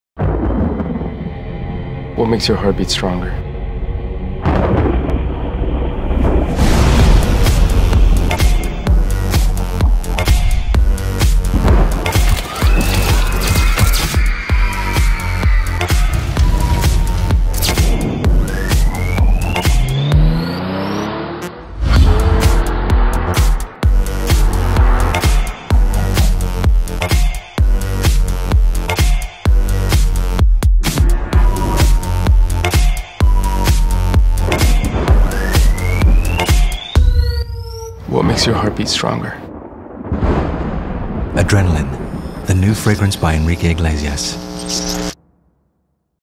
Enrique Iglesias Adrenaline férfi parfüm Parfümdivat hu
A népszerű énekes, Enrique Iglesias első illata, amely az Adrenaline Az Adrenaline a mandarin, a madagaszkári bors, a sáfrány, a citrom, a pirított tonkabab és különböző fűszerek keverékéből áll össze.

Még sohasem csináltam azt, amit vártak tőlem. Ez az illat a teljes élet, egy korlátok nélküliség, amikor nem bánunk meg egy döntést sem, és a szívünk gyorsabban ver. Az első illatom, az Enrique Iglesias Adrenaline pontosan ezeket az érzelmi pillanatokat közvetíti, amikor úgy érzem, igazán élek - nyilatkozta nemrégiben a tehetséges énekes. Nem kérdés, hogy Enrique Iglesias a modern zene egyik leginspirálóbb és legtehetségesebb művésze.

Bízunk benne, hogy az ő szenvedélye és hitelessége egy igazi illatenergiát hoz létre - mondta az együttműködésről a Coty INC vezérigazgatója, Michele Scannavini. Enrique Iglesias 2009 és 2013 között volt az Azzaro Man arca, ám az idei év elején úgy döntött, itt az idő, hogy saját parfümöt is a piacra dobjon. Januárban alá is írt egy együttműködési szerződést a Coty INC céggel.